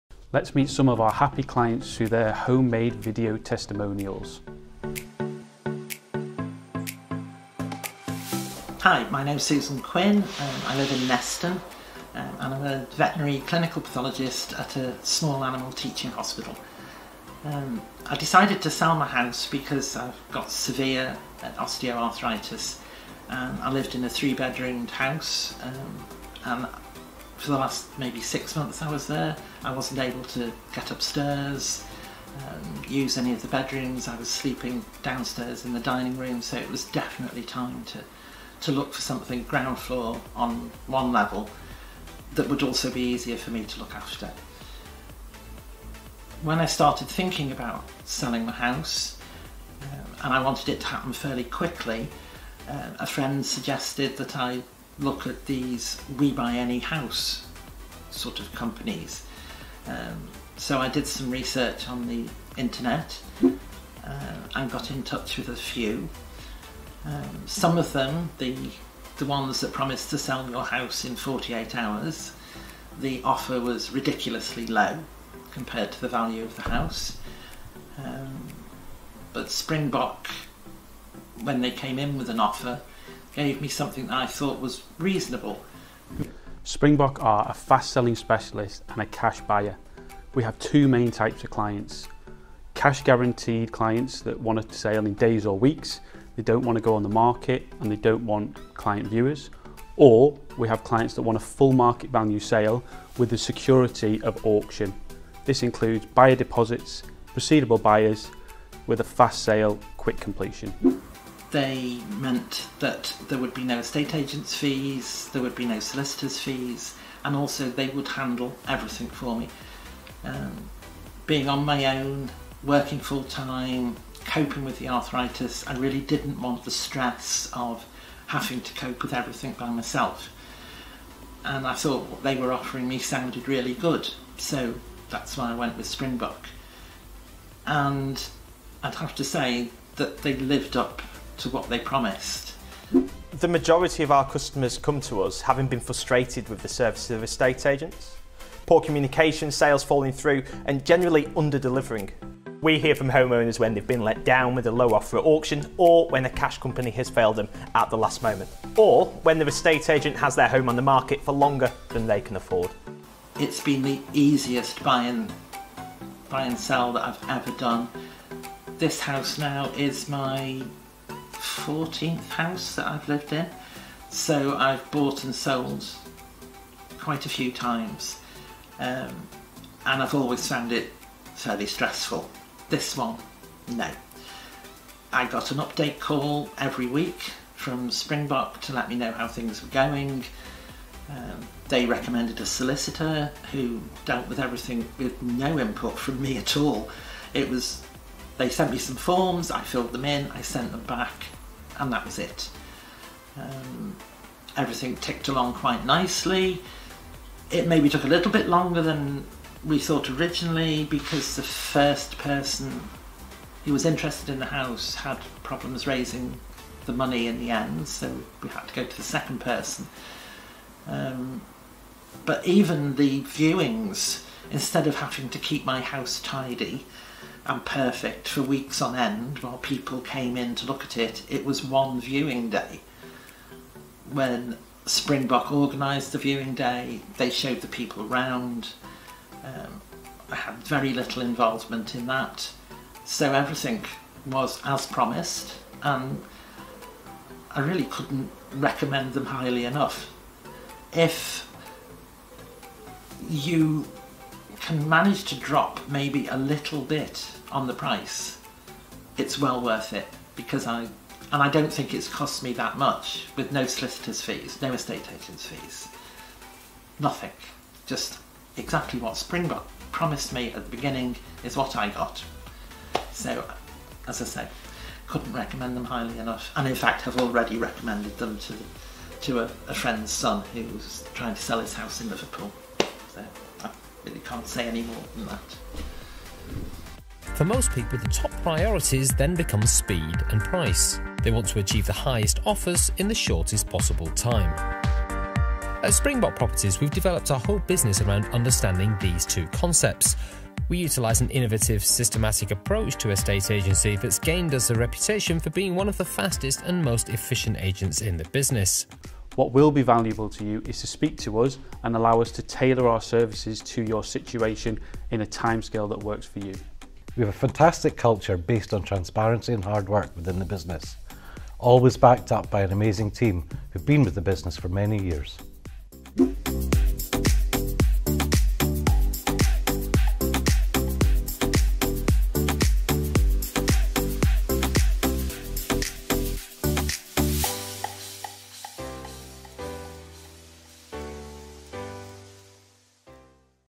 Susan is a veterinary clinical pathologist and she needed to sell her house for health reasons
Susan lives in a three bedroom house but needed to downsize because she couldn't go upstairs upstairs due to her severe osteoarthritis. Her friend recommended for her to look for a "We Buy Any House" companies and she decided that her best option would be Springbok Properties. The company will handle everything for her and she was offered the best price in the market for her property. Springbok Properties tailored their services based on Susan's situation.